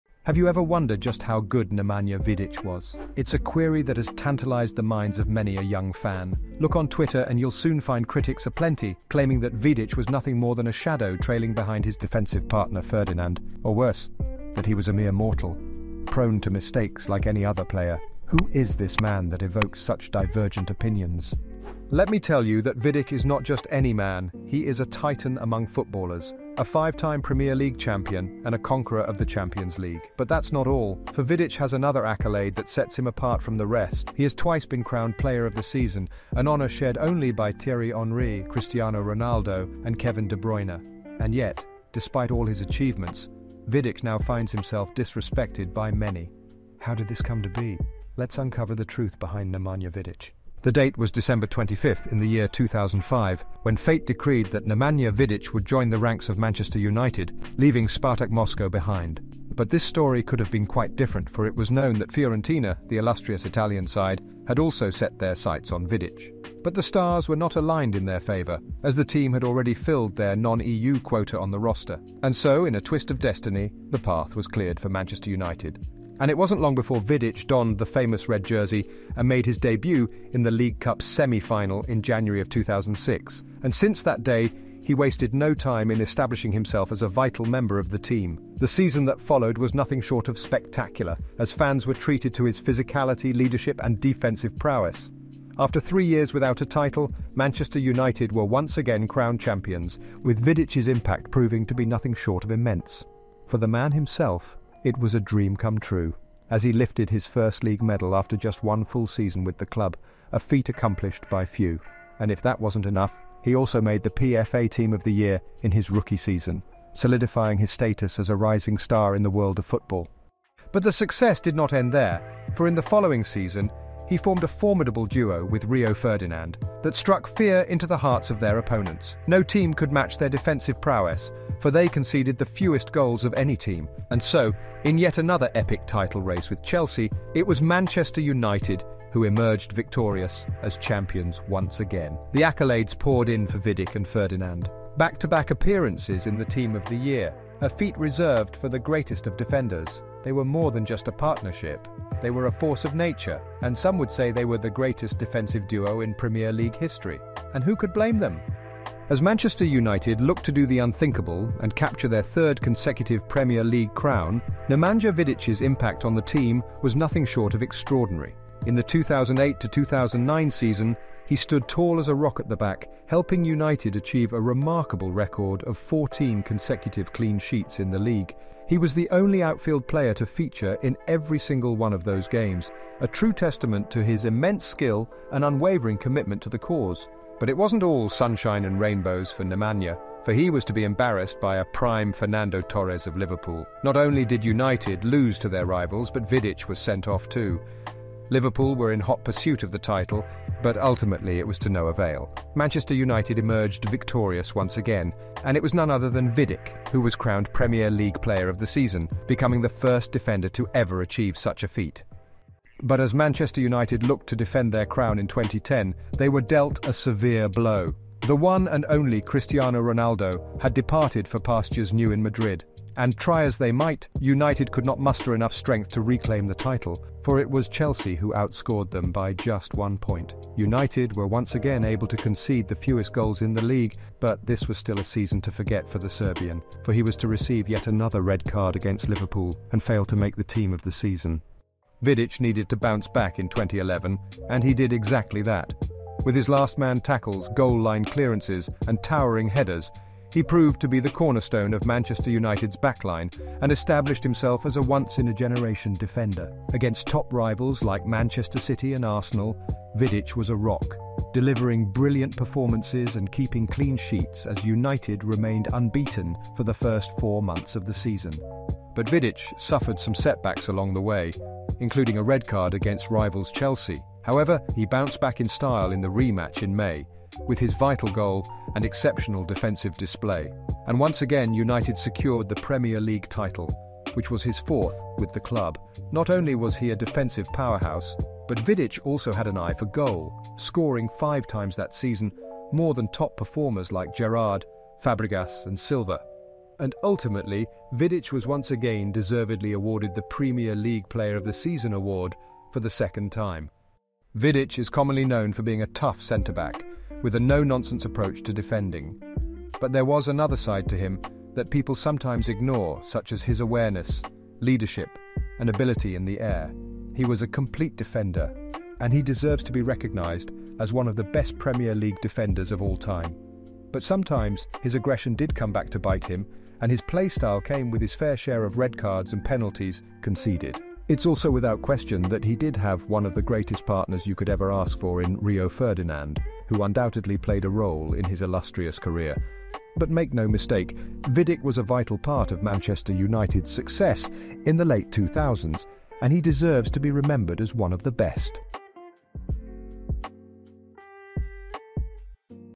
How Good was Nemanja Vidic?
Introducing the ultimate tribute to Nemanja Vidic, the legendary Serbian footballer who cemented his place as one of the greatest defenders of all time. This video delves into Vidic's illustrious career, highlighting his impressive leadership, tenacity, and unyielding dedication on the pitch.

Join us as we explore Vidic's rise to stardom, his unforgettable partnerships with fellow Manchester United teammates such as Rio Ferdinand, Patrice Evra, and Wayne Rooney, and the tactical genius of his legendary manager Sir Alex Ferguson.

This video offers an in-depth look at the career of Nemanja Vidic, his unparalleled ability to read the game, and his numerous accolades and achievements, including five Premier League titles, a UEFA Champions League triumph, and numerous individual awards.

Don't miss out on this exciting opportunity to learn more about one of the most dominant and respected defenders in football history. Boost your knowledge of the beautiful game and gain a newfound appreciation for the incredible legacy of Nemanja Vidic.